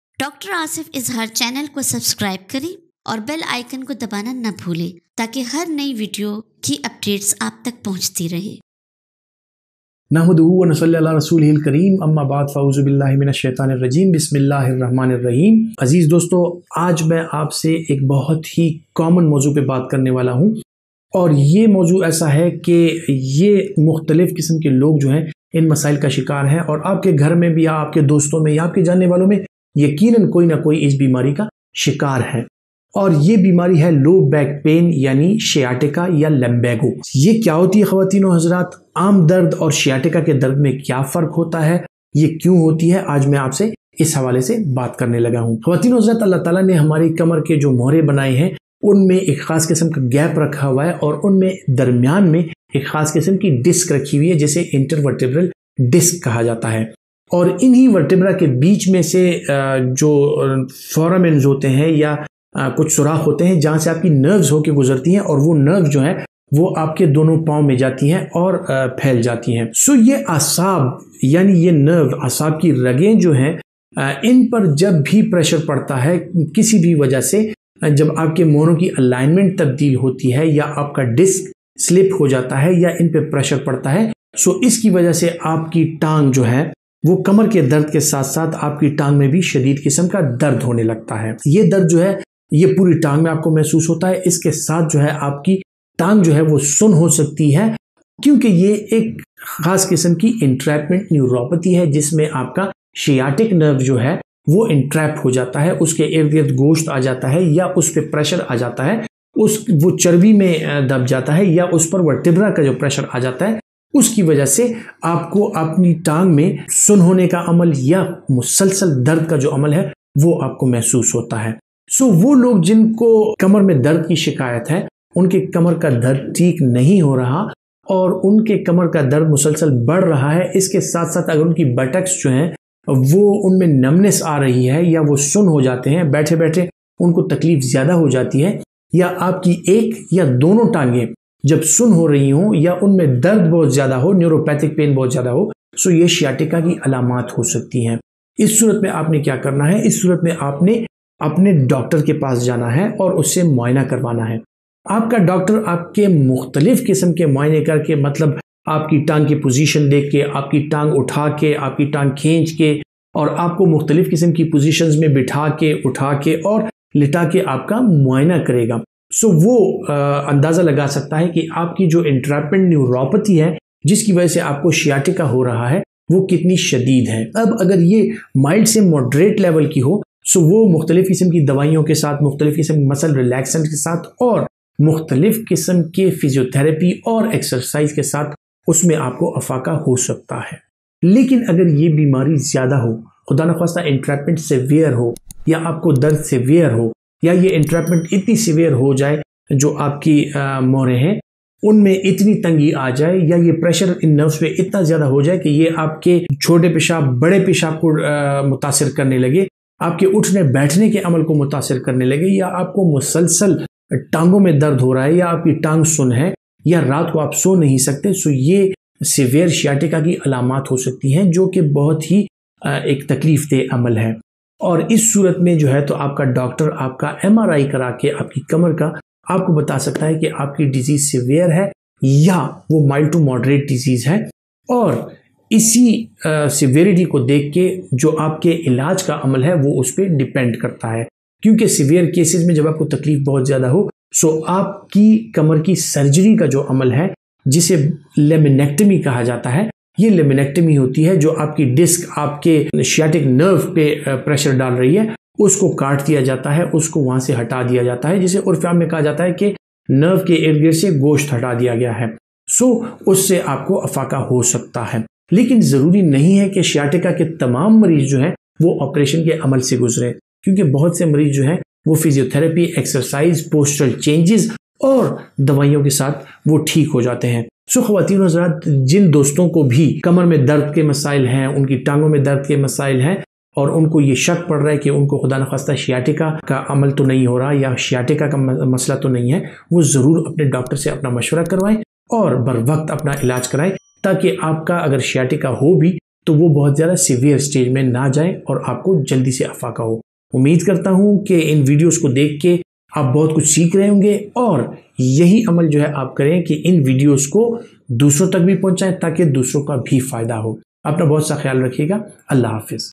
Sciatica Pain Relief In Urdu/Hindi | Kamar Dard Ka Ilaj | Kamar Dard Treatment
Sciatica is one of the most common neurological conditions, and it can be really painful. It is a crippling pain that can come from any number of causes. One of the most common causes is sitting for too long. This is a condition that affects the sciatic nerve, which runs from your back down to your legs. You can get relief from sciatica through stretching, massage, and anti-inflammatory medication. But the best way for anyone to get relief from sciatica pain is by seeking medical treatment.